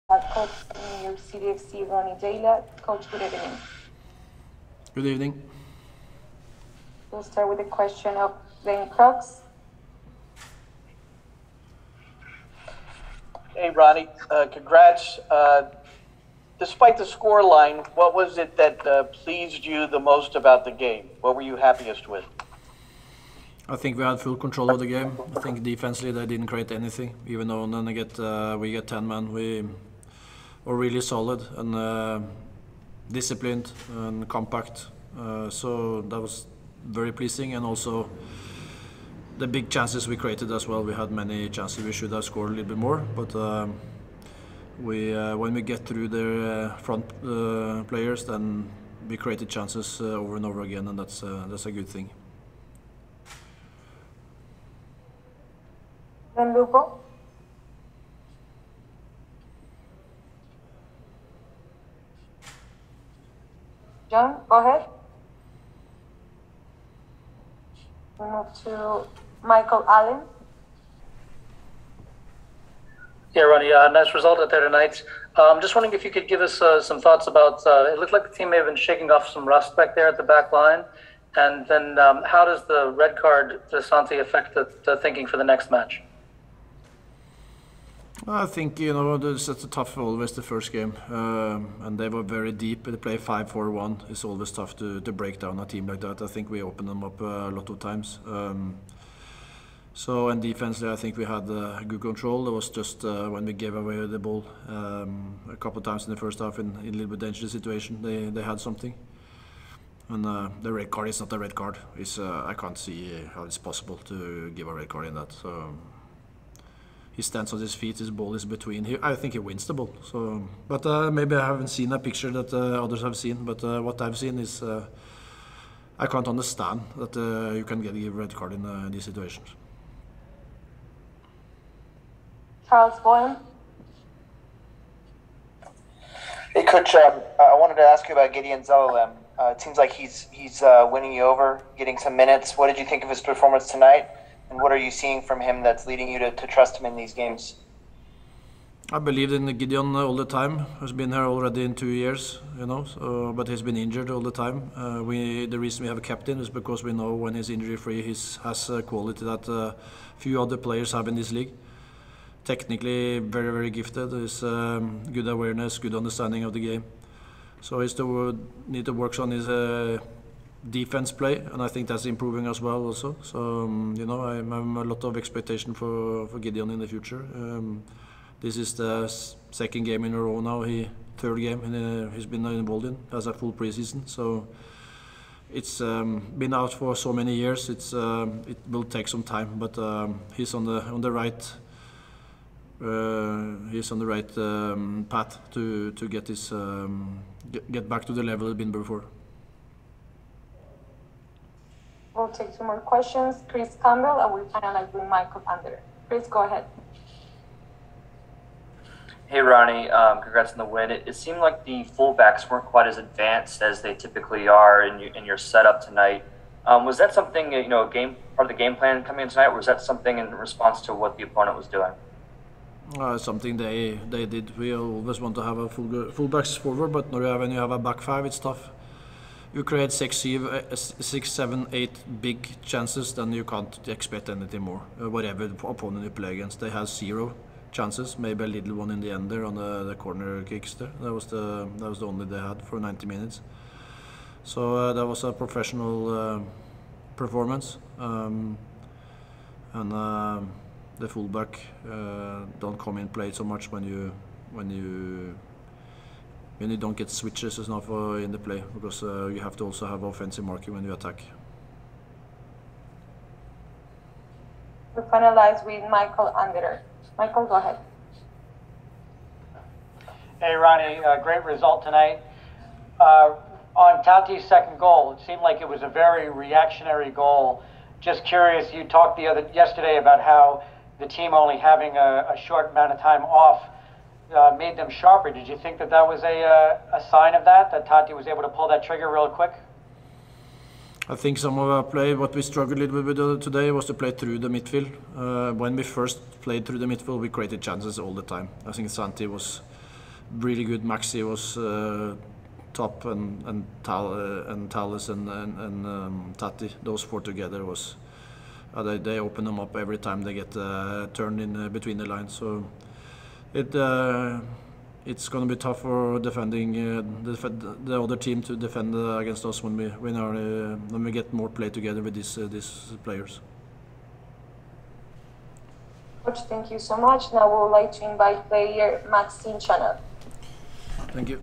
Ronny Deila | Santos de Guápiles 0-2 NYCFC
New York City FC Head Coach Ronny Deila speaks to the media after the Club's 2-0 win against Santos de Guápiles on Tuesday, February 15, 2022.